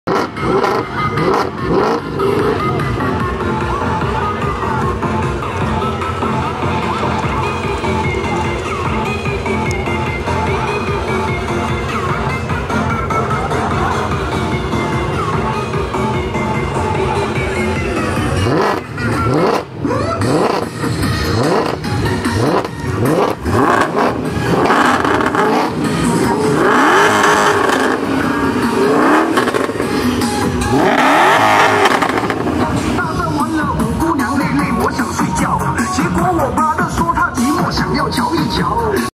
20200829 161432 Benz C63後驅車比賽前要先燒胎，以達到輪胎工作溫度
後驅車比賽前要先燒胎，以達到輪胎工作溫度